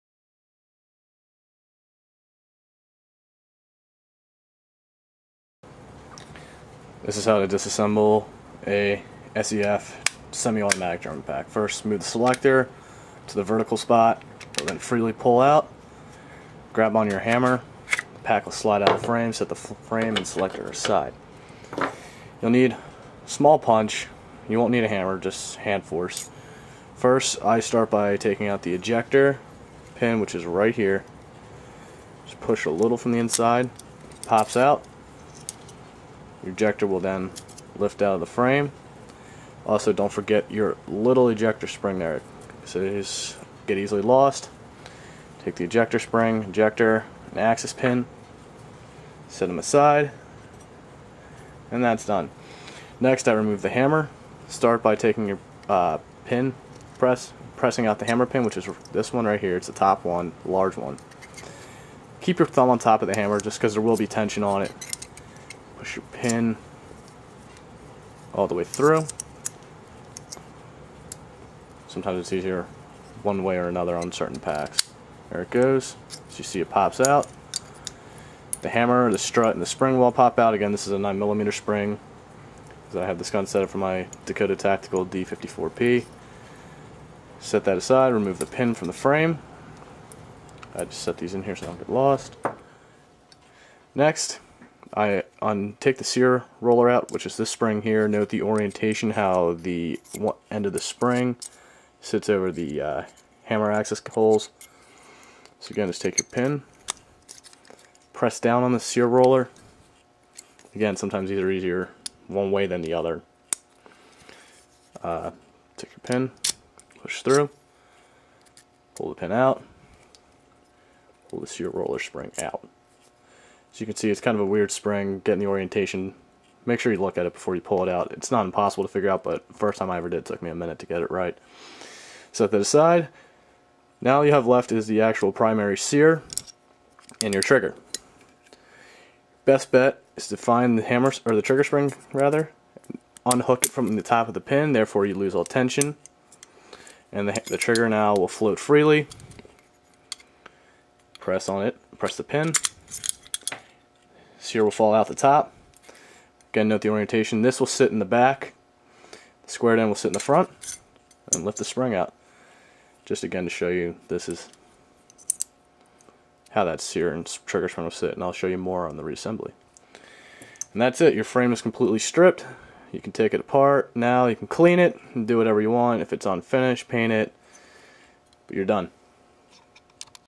H&K Semi Auto SEF Pack Disassembly
Video guide on how to disassemble a H&K 9x series trigger pack (semi only),